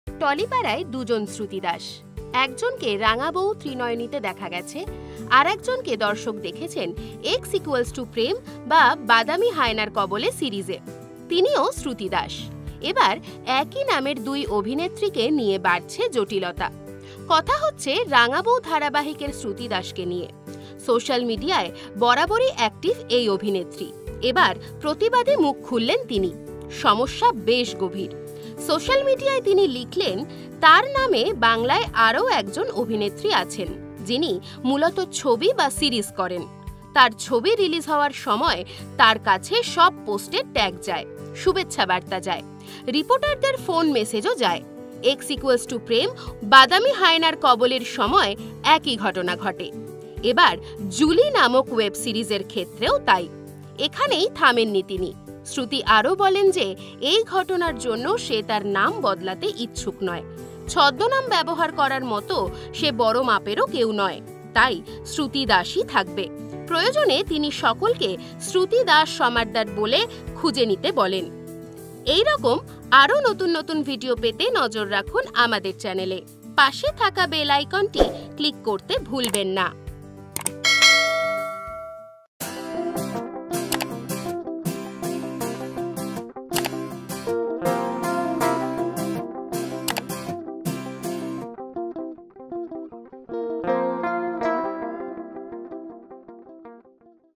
একই নামের দুই শ্রুতি দাস কে নিয়ে বাড়ছে সমস্যা। Shruti Das । Interview । Name confusion advice
টলি পাড়ায় দুজন শ্রুতি দাস। একই নামের দুই অভিনেত্রী কে নিয়ে বাড়ছে জটিলতা। রাঙা বৌ ধারাবাহিকের শ্রুতি এবার  প্রতিবাদে মুখ খুললেন। সমস্যা বেশ গভীর।

Shruti Das Exclusive Interview: Clearing the Air on Name Confusion